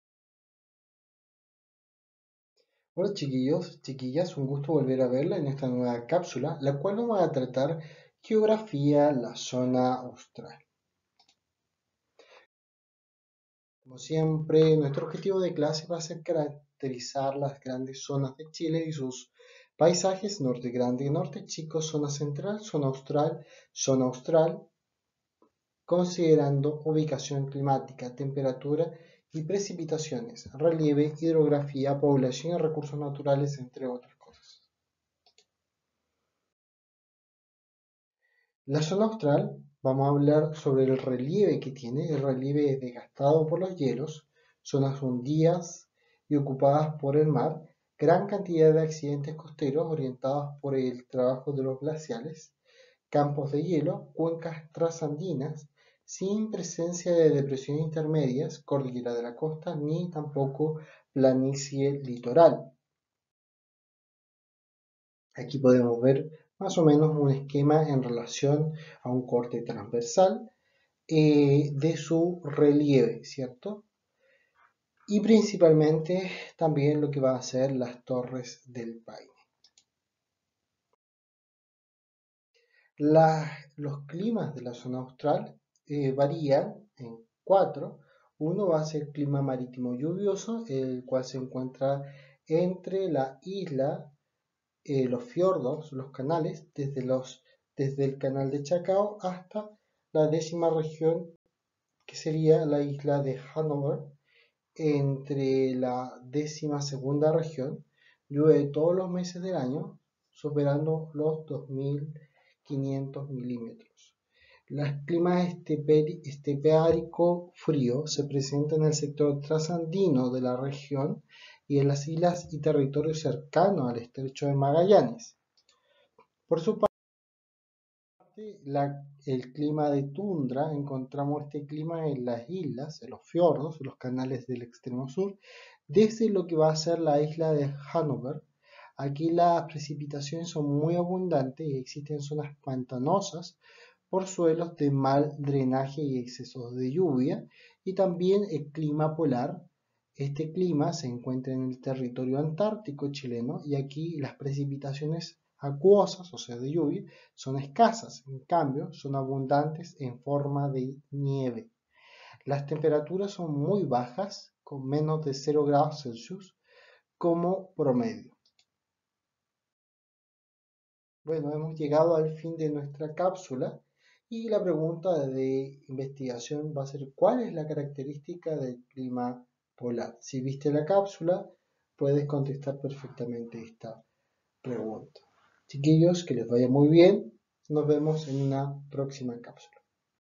Historia 5° 2021 Cápsula de Geografía austral semana del 15 de noviembre
Cápsula de Aprendizaje Historia, Geografía y Ciencias Sociales 5° años Escuela Calicanto, semana del 15 al 19 de noviembre de 2021